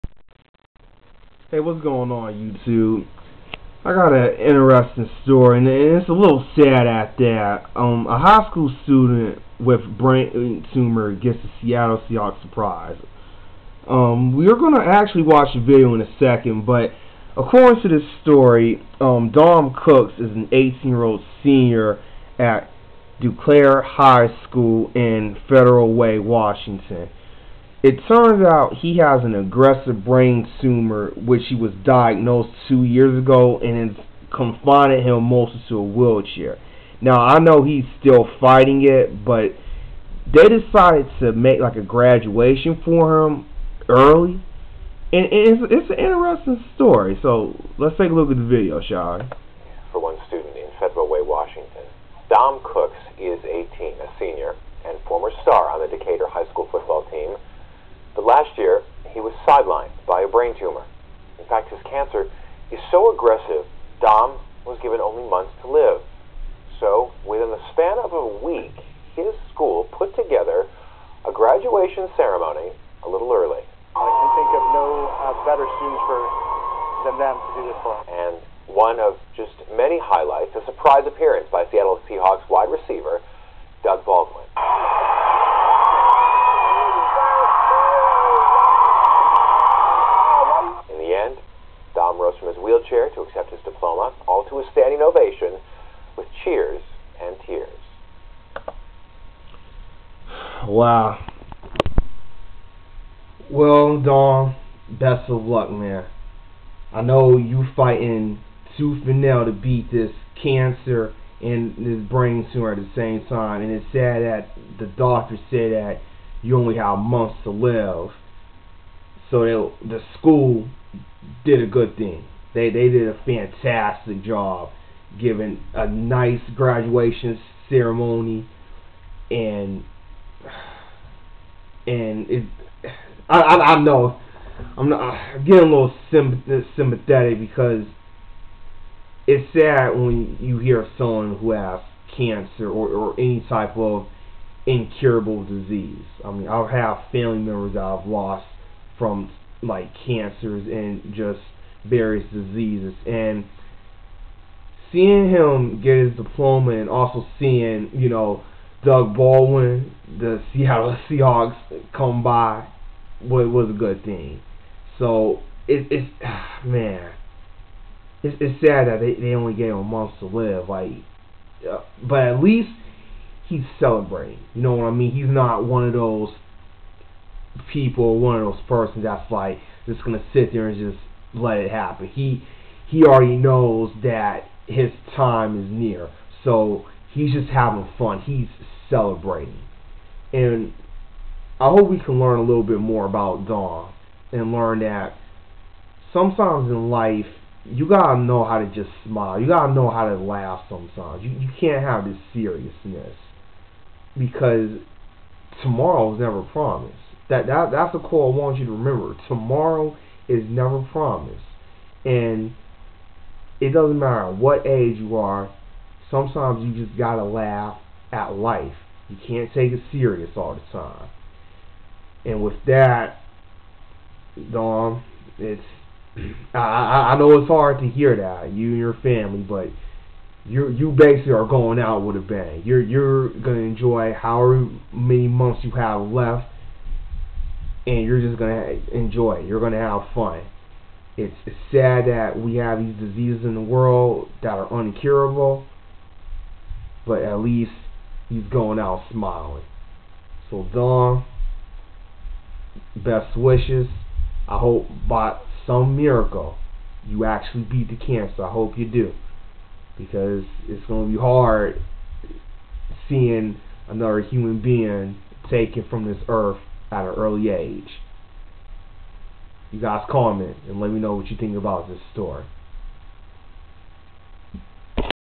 High School Student With Brain Tumor Gets Seattle Seahawks Surprise
live everyday like its you last